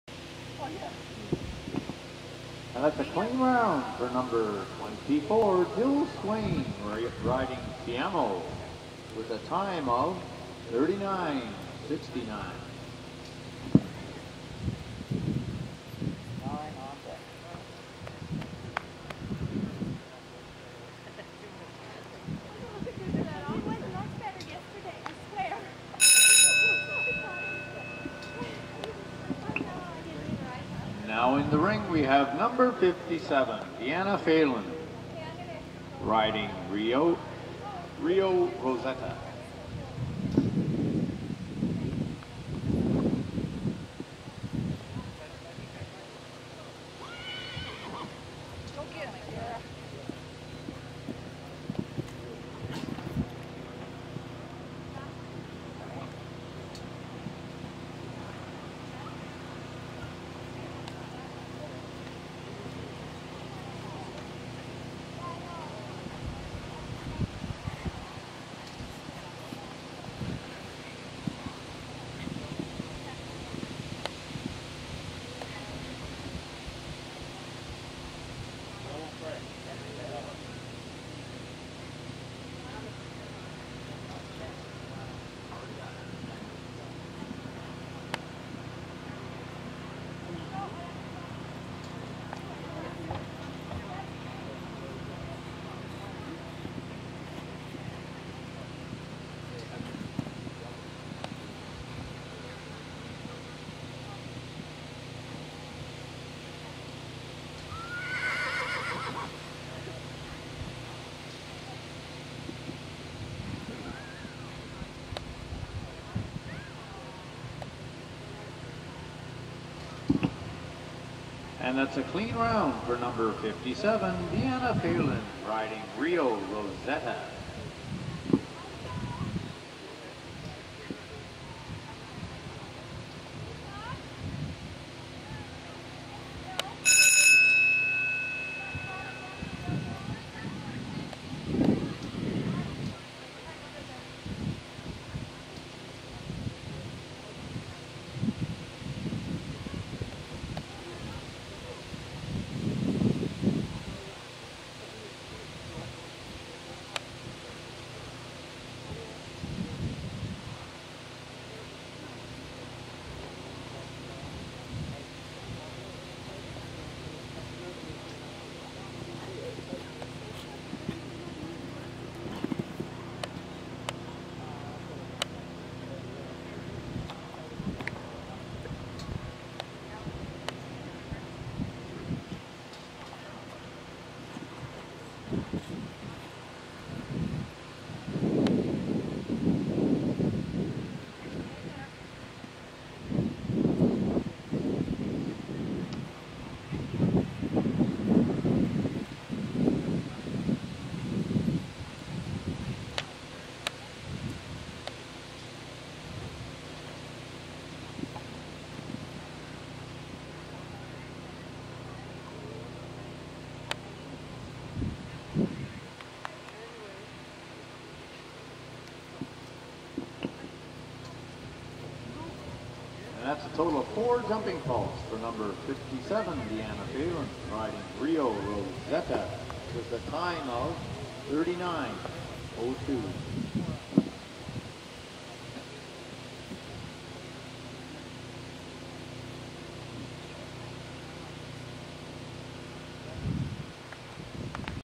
Rio Rosetta - Rolling Hills July 2009 - 1m 15 - m2ts format
Rio Rosetta ridden by Deanna Phelan in the July 18 2009 Rolling Hills 1m 15 Jumper Class.  Tied for  Champion.